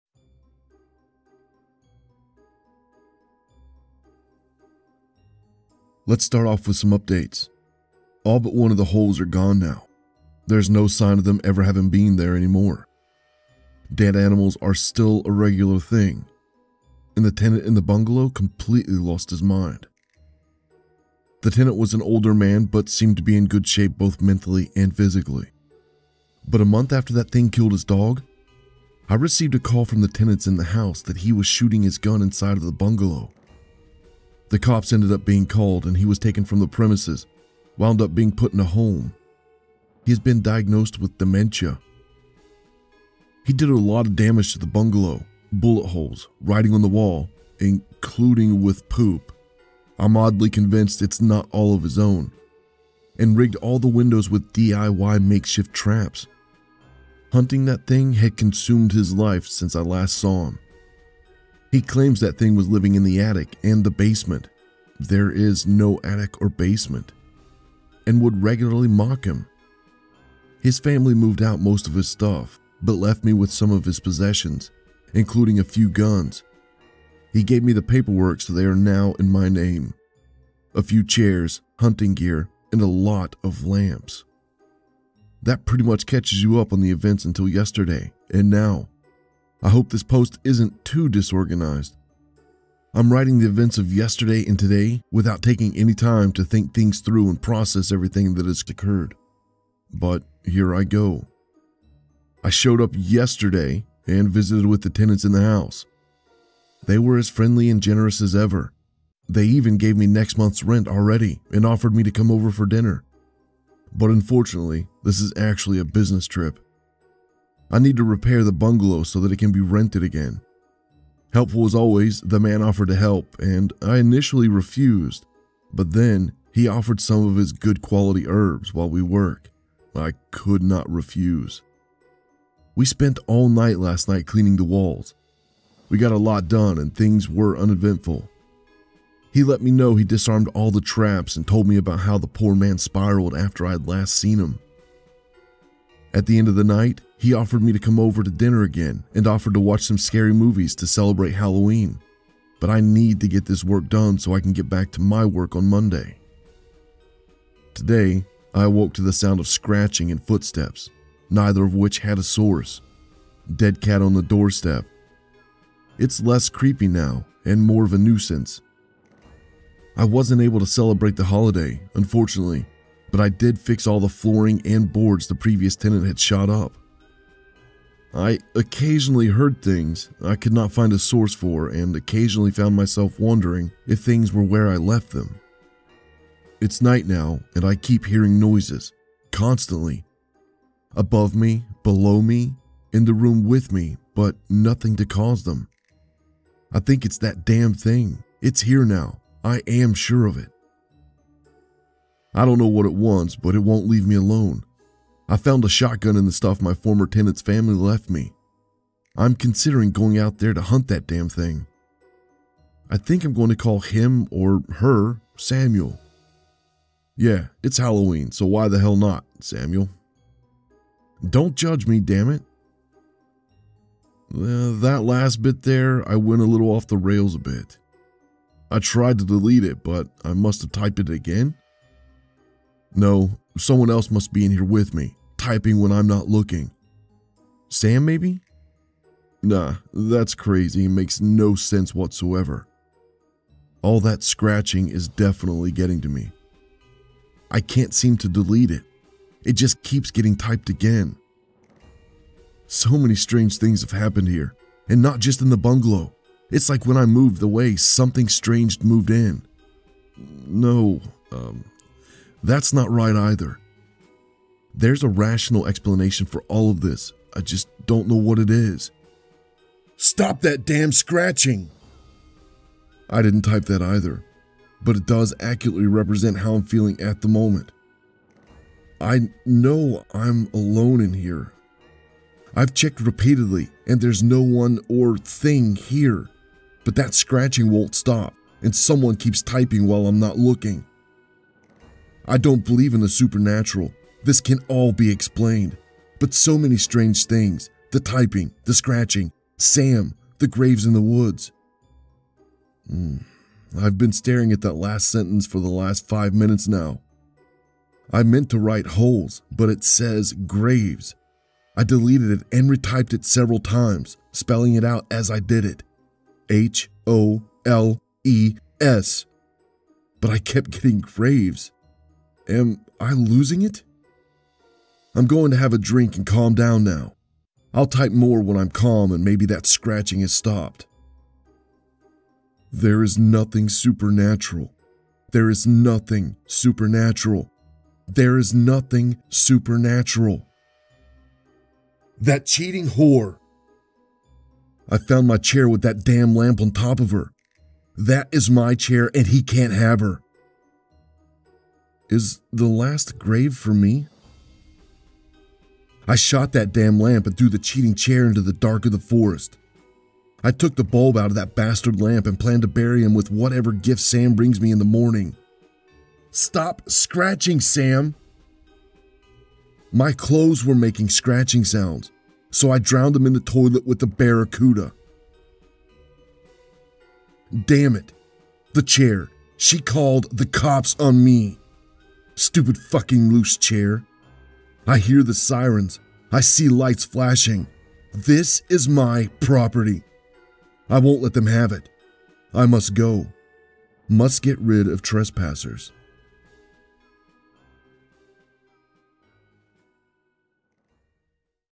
SCARY Halloween Storytime - I am spending Halloween at the bungalow by: u/uLasalle8 - Day 18
What happens in the dark of the woods when there are ghosts about?

                                        * *     Audio/Image Credits    * *